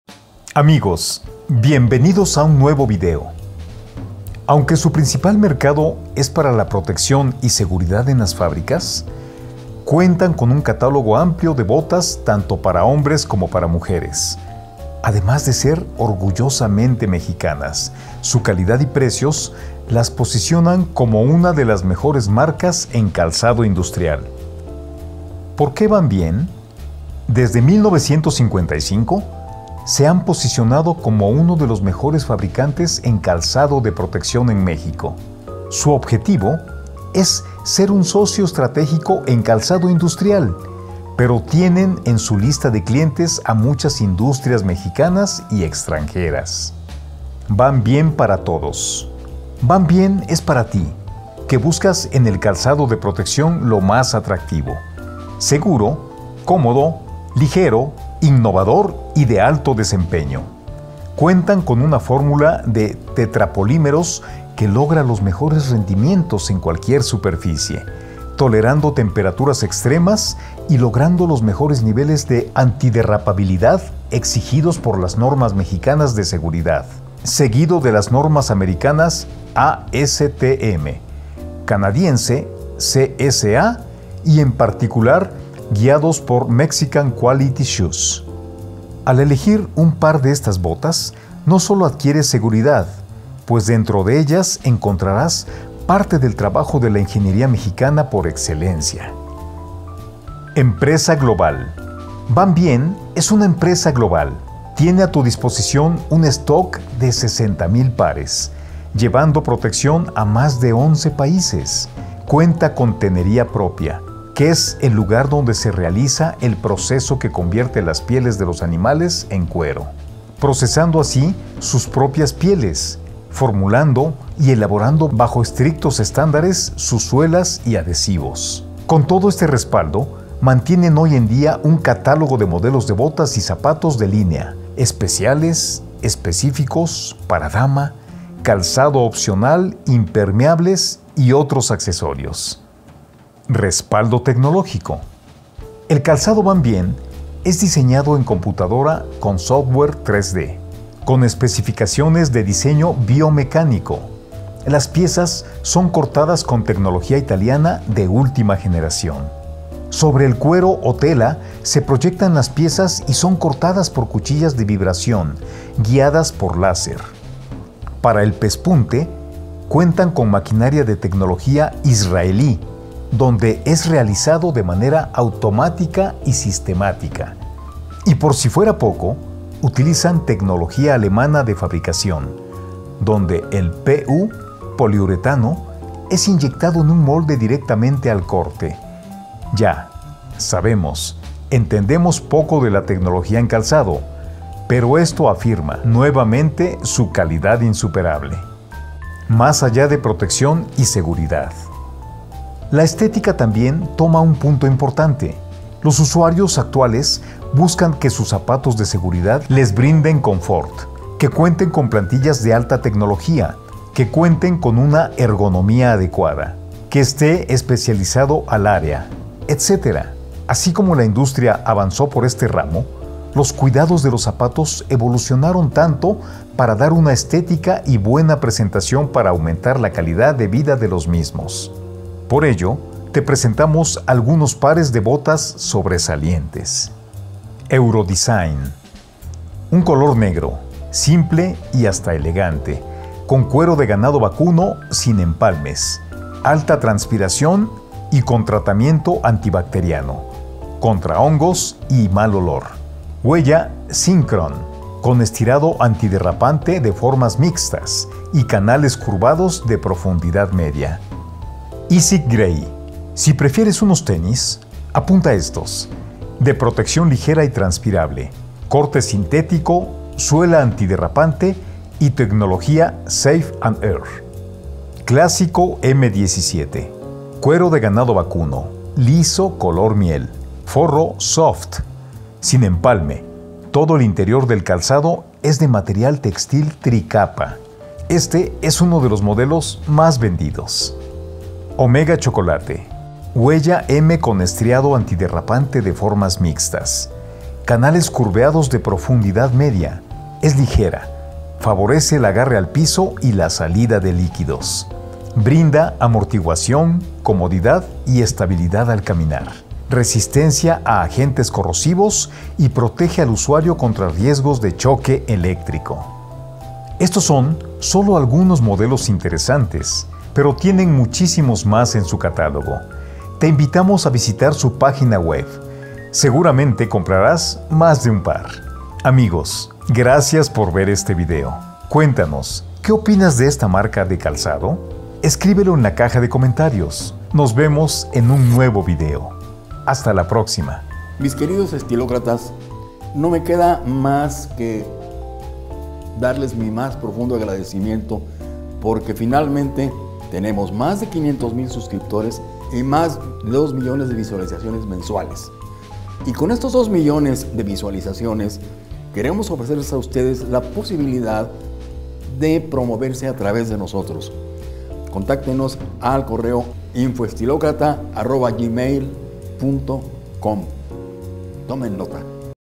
BOTAS VANVIEN: POR QUÉ DESPUNTARON TAN DE PRONTO PARA HOMBRES, ¿LAS CONOCES?
ESTA MARCA HA GANADO A PULSO SU FAMA, PERO SABES POR QUÉ, QUIZÁ NO SABÍAS ESTO. SI TE GUSTAN LAS BOTAS VE ESTE VIDEO

00:00 Intriducción
00:25 ¿Porqué Vanvien?
00:47 Vanvien para todos
01:40 Empresa global
02:23 Respaldo tecnológico
03:20 Más allá de protección y detalles
03:58 Modelo Eurodesign
04:23 Modelo Isik Gray
04:37 Modelo Clásico M17
04:52 Modelo Omega Chocolate

Visita nuestro sitio asociado de Amazon para productos de tu interés: